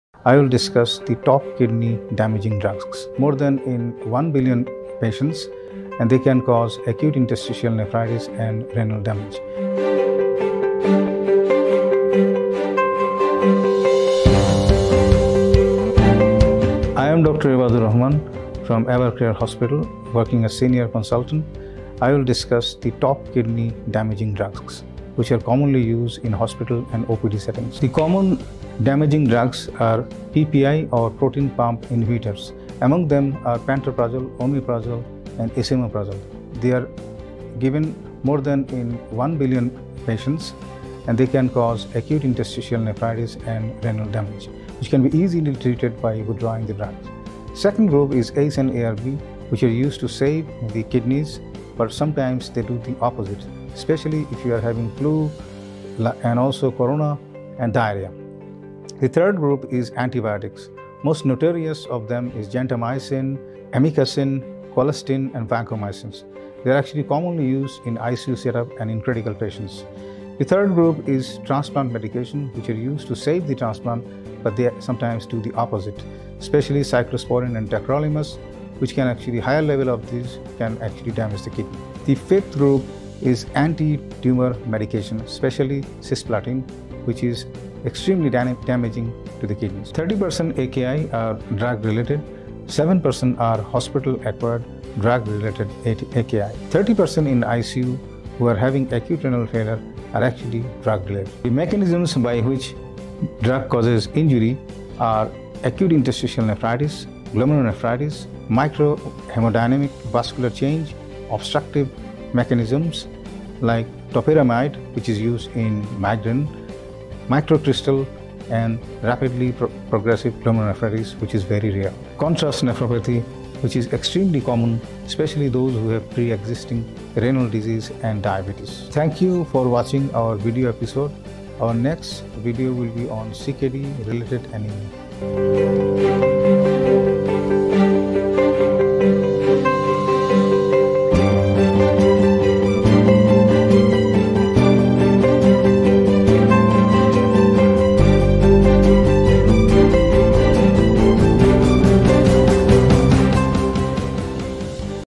Dr. Ebadur Rahman on kidney injury
Dr. Ebadur Rahman, FRCP (Edin), FASN, MRCP (UK), DIM (UK), serves as Senior Consultant of Nephrology at Evercare Hospital Dhaka. This expert nephrologist speaks about drugs that can damage one’s kidney. He also mentions the mechanisms which may cause such injuries. Watch this video for detail.
For appointments and consultations, dial 10678.